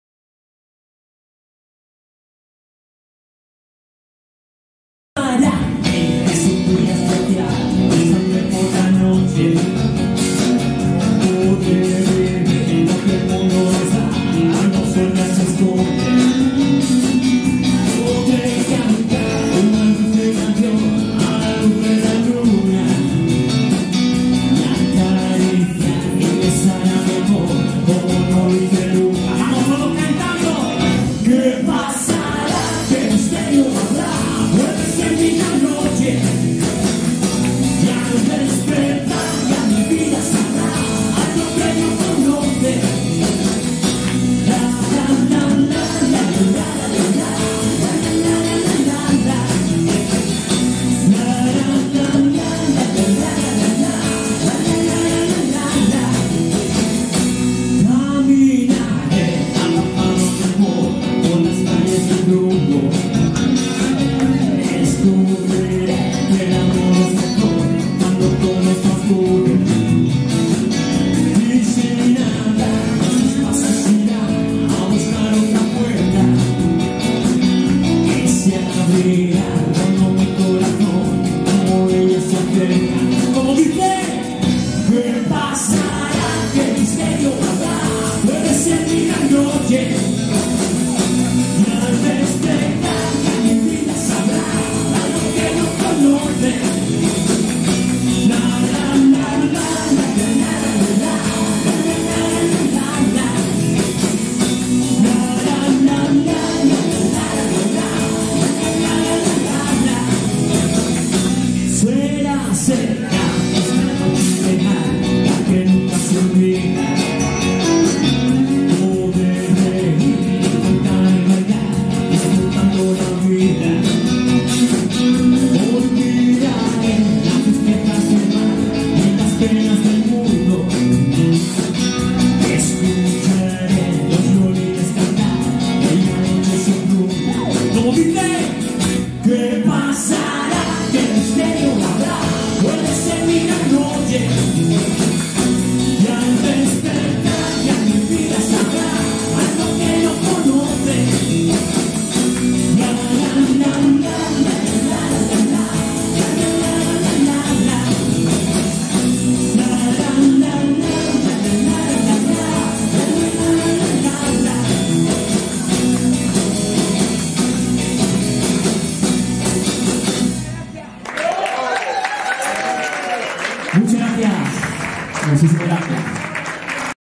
Concierto CALLE EL PEÑÓN en A.C. La Guitarra Poética 9.18
Concierto de CALLE EL PEÑÓN en A.C. La Guitarra Poética de Conil de la Frontera (Cádiz), celebrado el 1 de mayo de 2021.